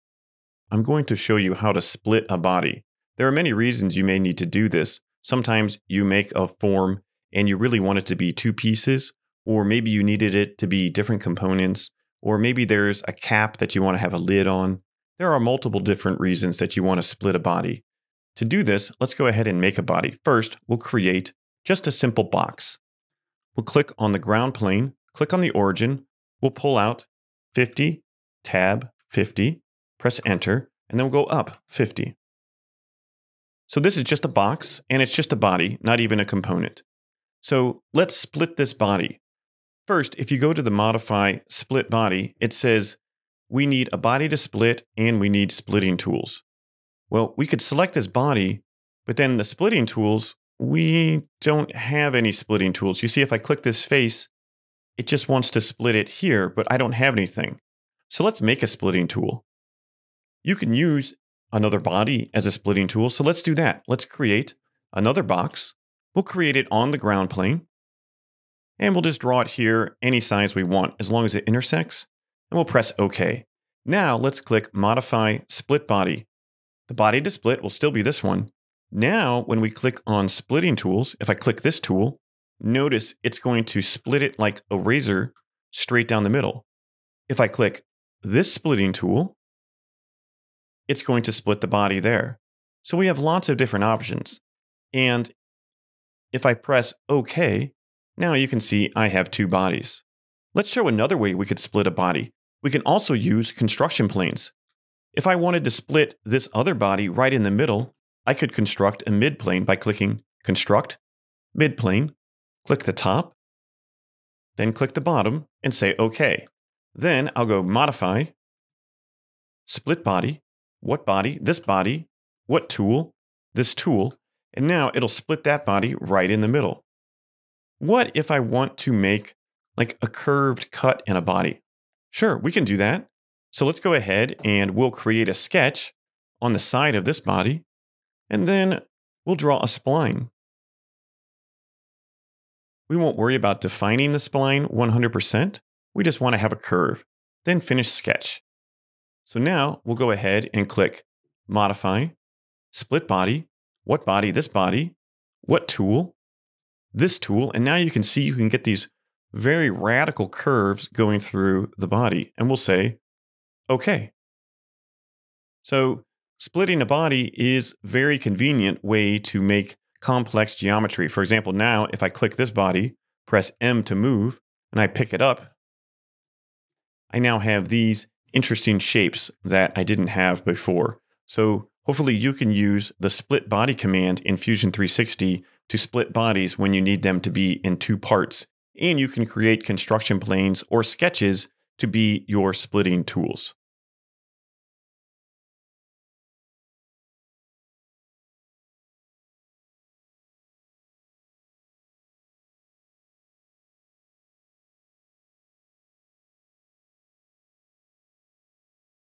How to Split a Body in Fusion 360 Tutorial
How to split a body in Fusion 360 with straight or curved tools.